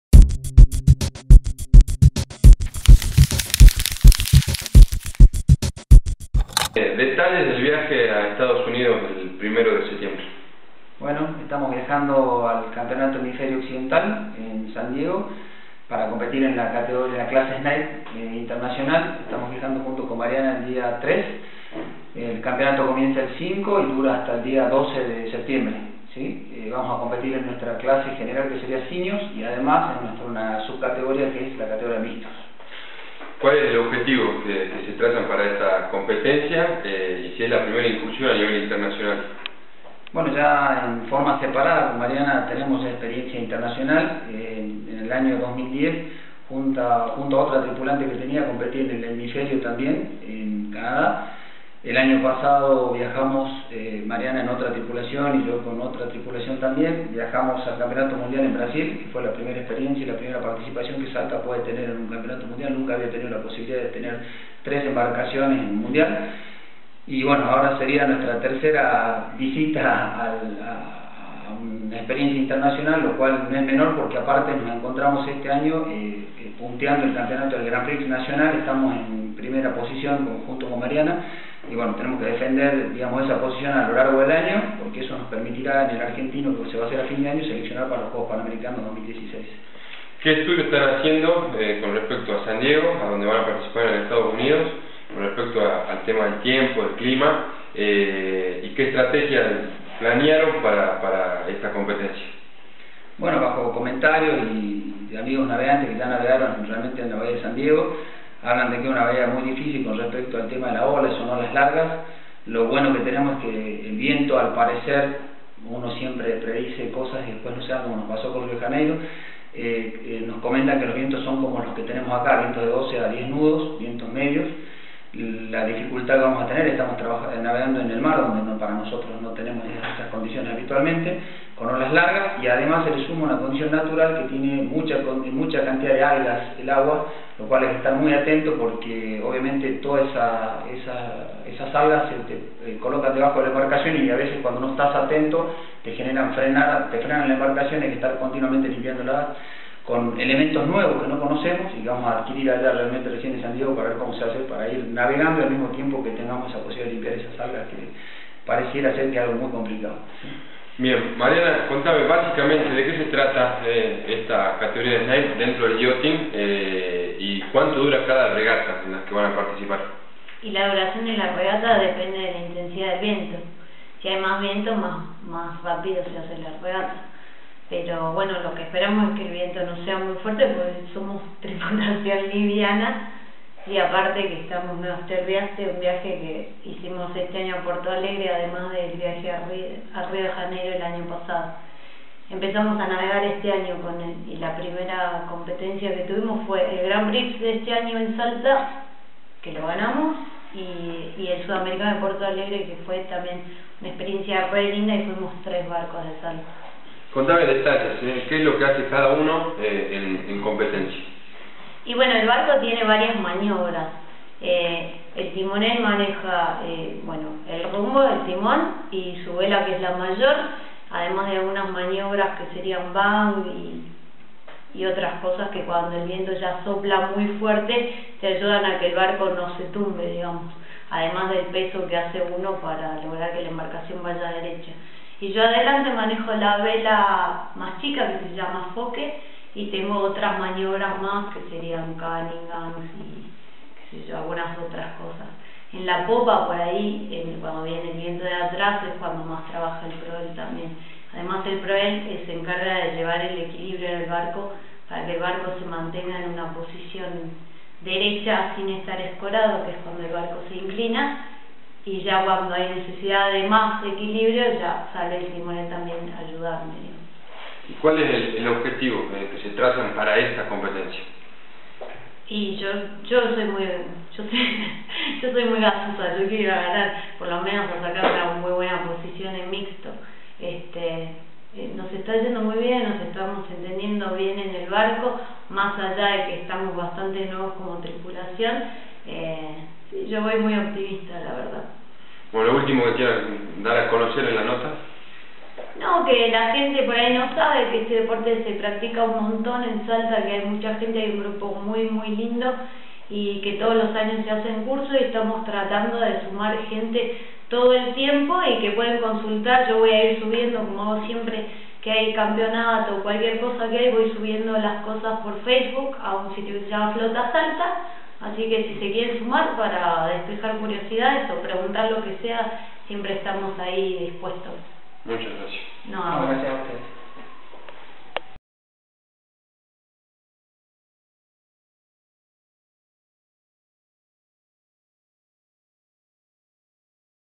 Yachting: Mariana Safar y Alejandro Triggiano
Entrevista a Mariana Safar y Alejandro Triggiano en La Gaceta Salta